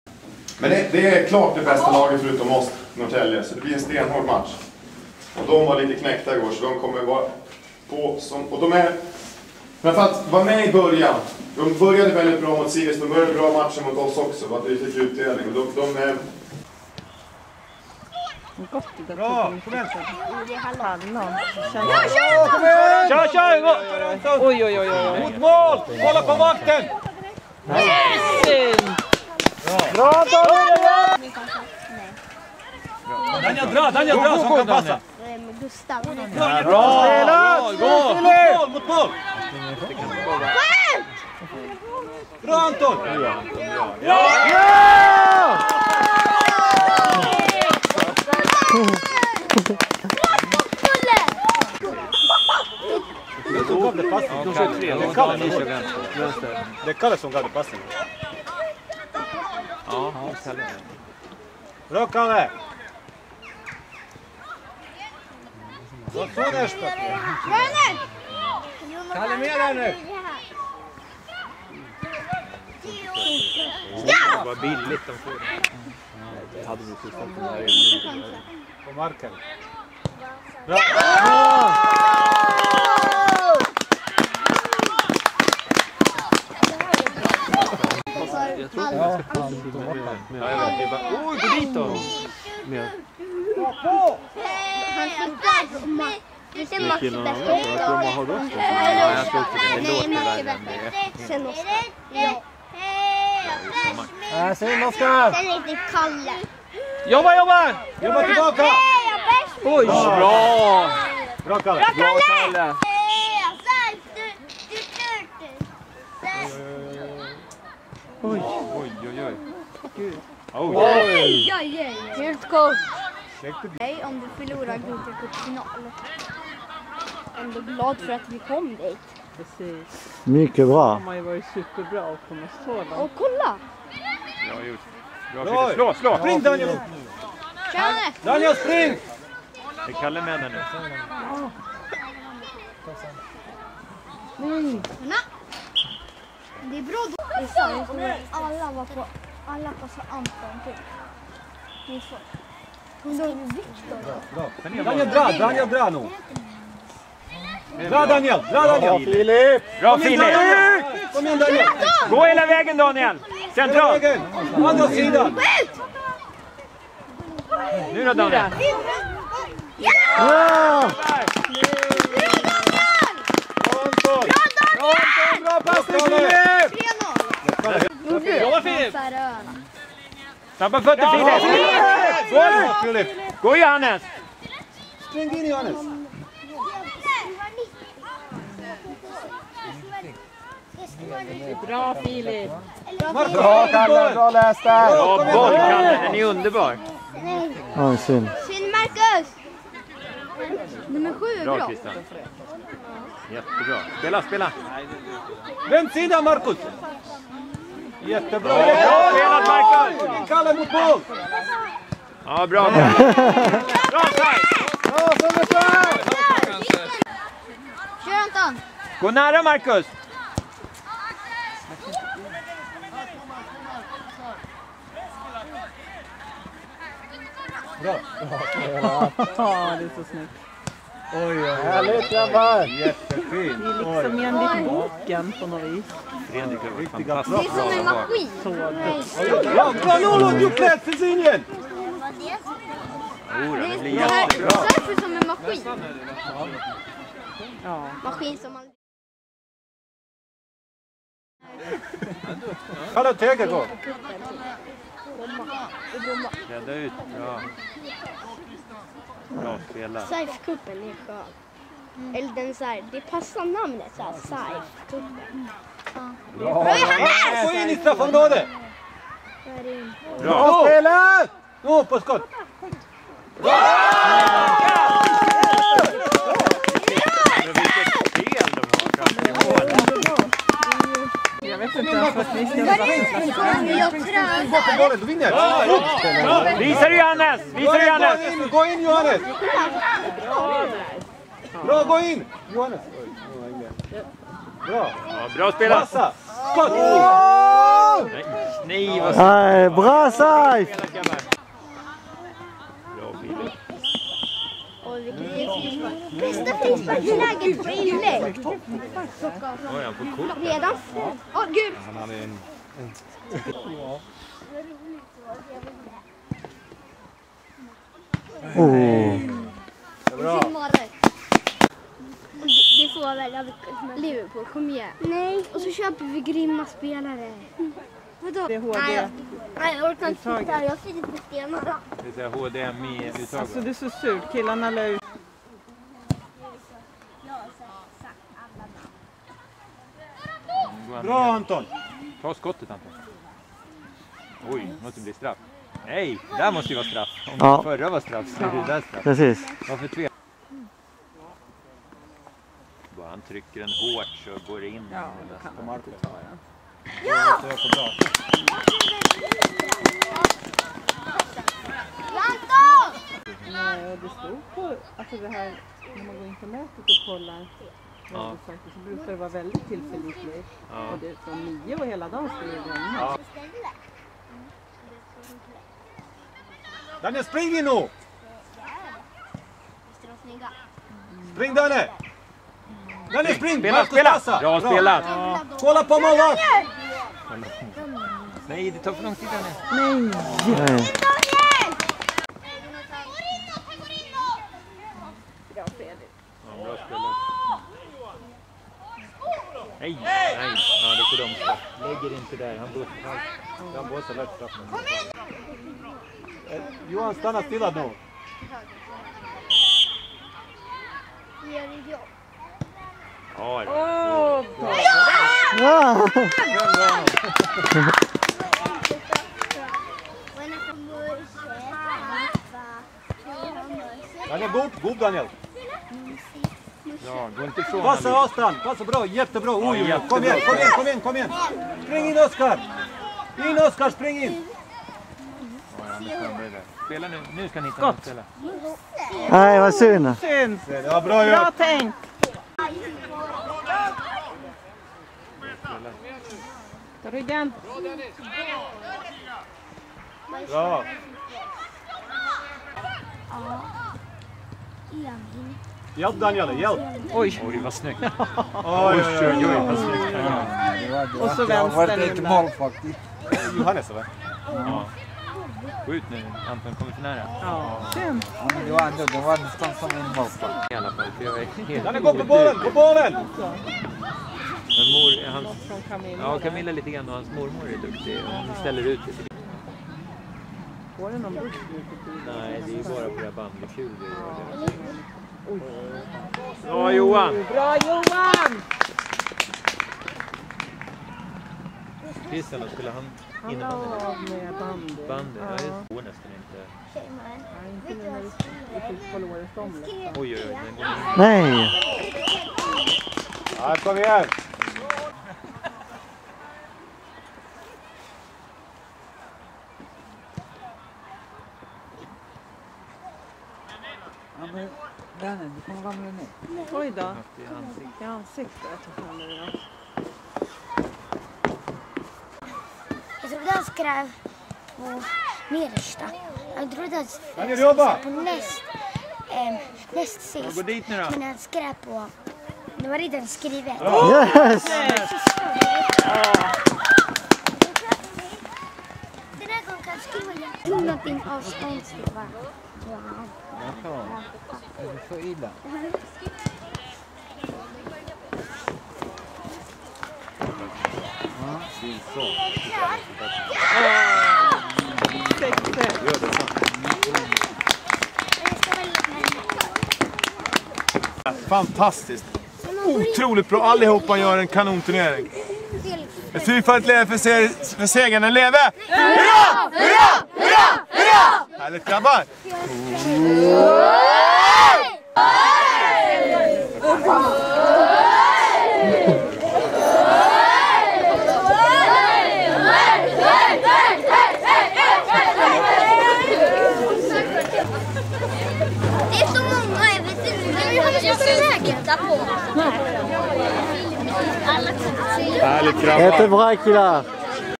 SAIF P98 - BKV Norrtälje P97
Vi representerade SAIF P97 i Håbo höstcup P97 med vårt P98-P99 lag.
Vi vann alla matcher, bland annat finalen mot BKV Norrtälje P97.